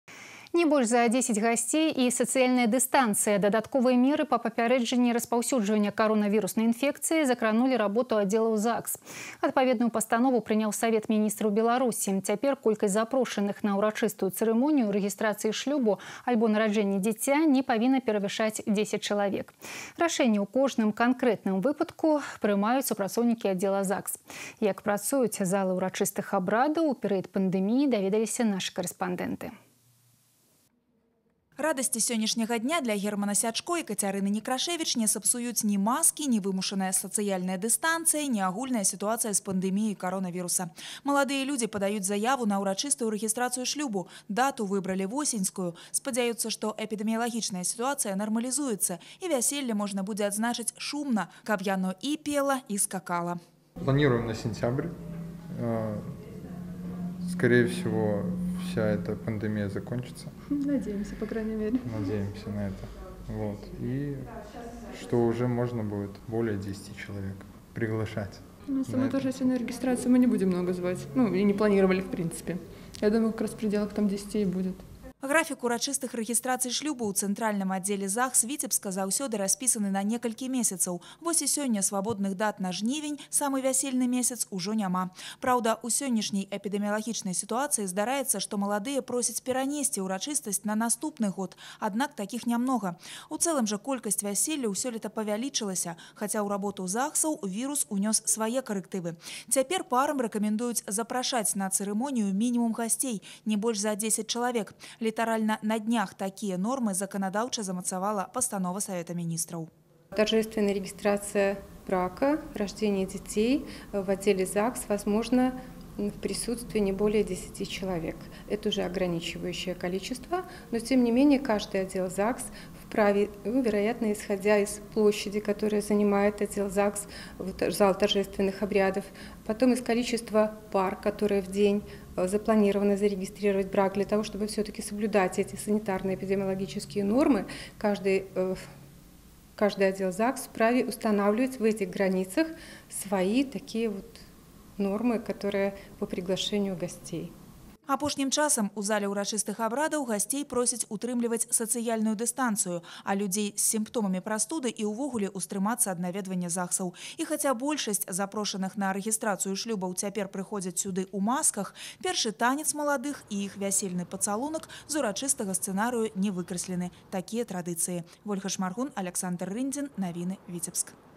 Как работают ЗАГСы Витебска в период пандемии? (27.05.2020)
В период коронавирусной пандемии изменилась условия работы многих организаций. Внесены коррективы и в деятельность ЗАГСов. Торжественные мероприятия проходят с соблюдением новых правил.

Ольга Шмаргун с подробностями.